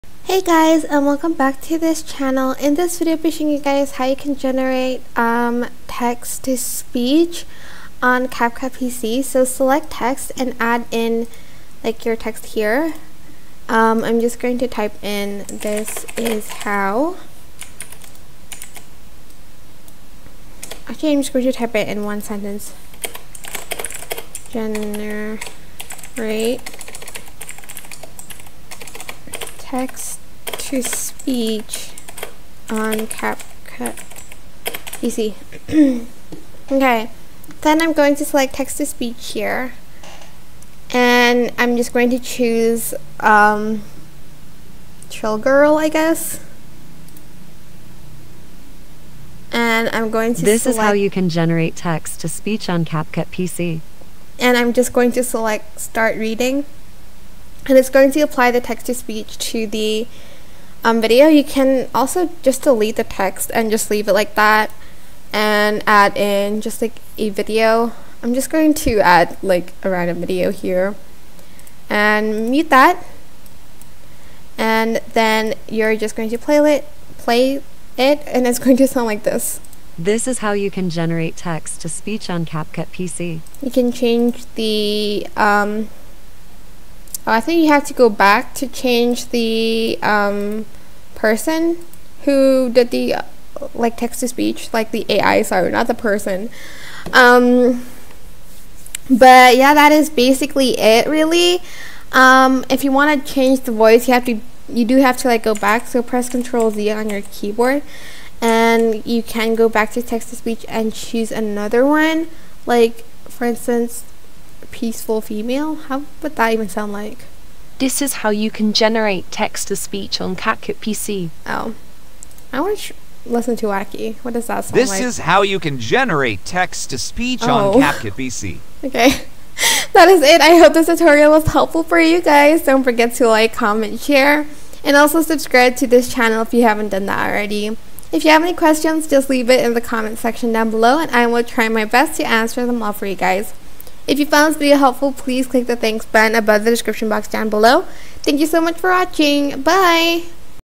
How To Generate Text To Speech In CapCut PC
Learn how you can generate text to speech in CapCut PC by watching this video.